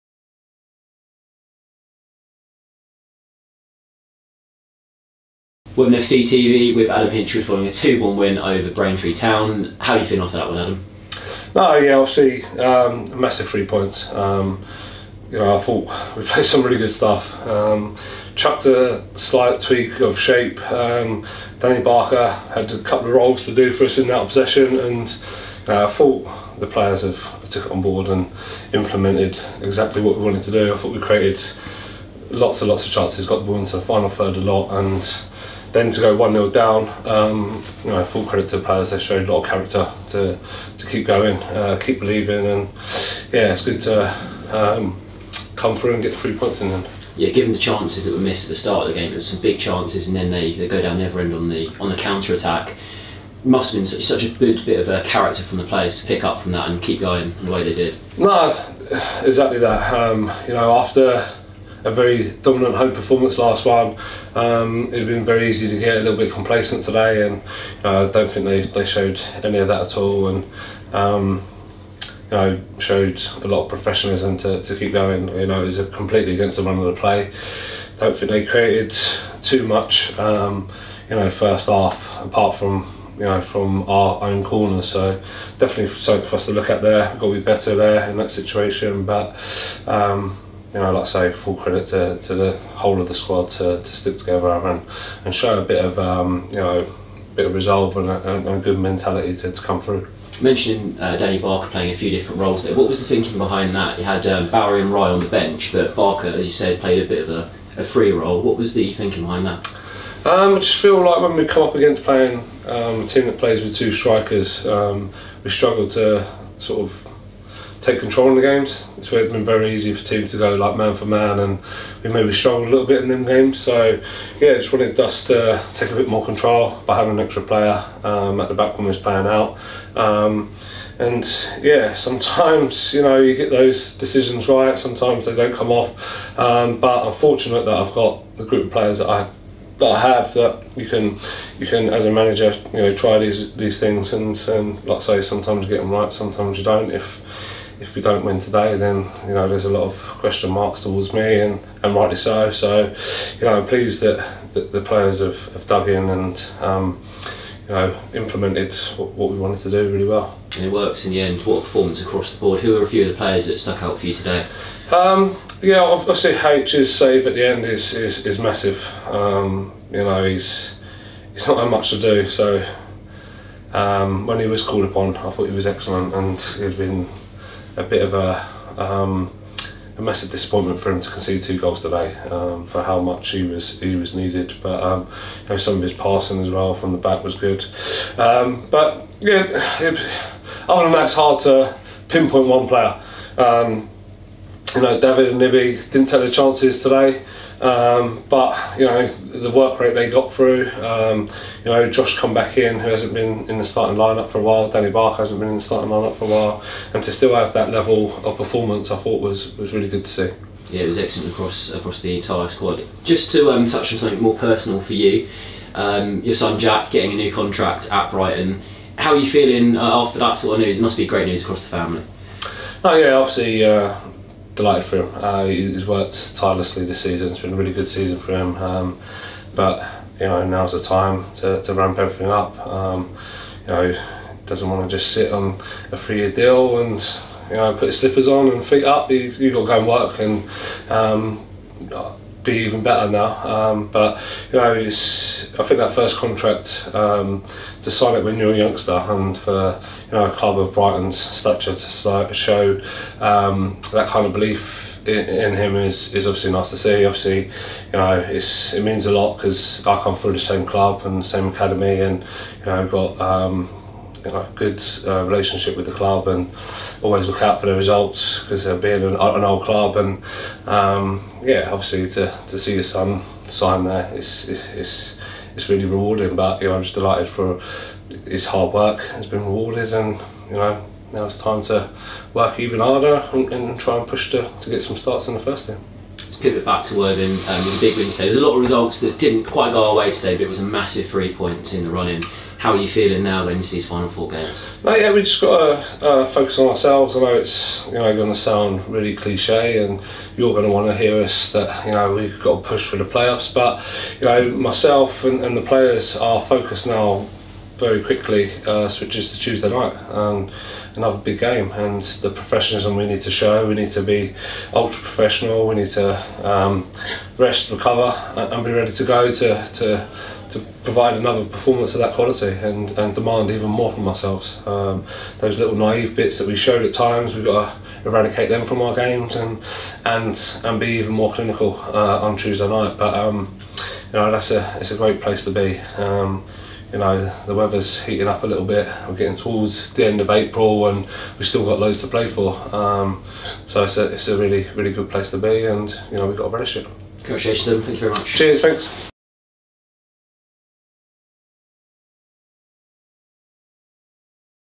"We need to be ultra professional" | Adam Hinshelwood | Braintree Town Reaction
Manager Adam Hinshelwood gives his thoughts on the 2-1 home win against Braintree Town in the National League South.